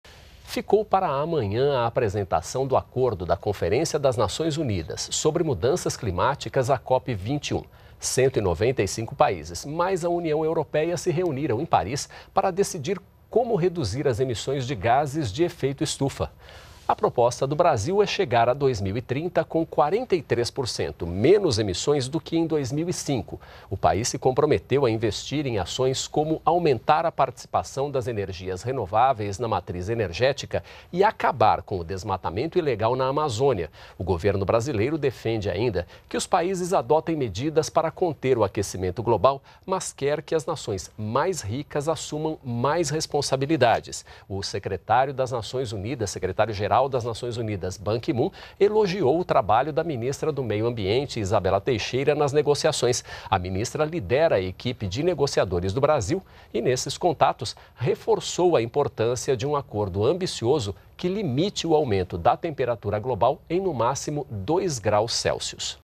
Acordo final da Conferência do Clima será divulgado neste sábado
NBR NOTÍCIAS - 11.12.15: O acordo final da Conferência das Nações Unidas Sobre Mudanças Climáticas (COP21), realizada em Paris, será divulgado neste sábado (12). A proposta do Brasil é chegar a 2030 com 43% menos emissões do que em 2005. O país se comprometeu a investir em ações, como aumentar a participação das energias renováveis na matriz energética e acabar com o desmatamento ilegal na Amazônia. O governo brasileiro defende ainda que os países adotem medidas para conter o aquecimento global, mas quer que as nações mais ricas assumam mais responsabilidades. O secretário-geral das Nações Unidas, Ban Ki-Moon, elogiou o trabalho da ministra do Meio Ambiente, Izabella Teixeira, nas negociações.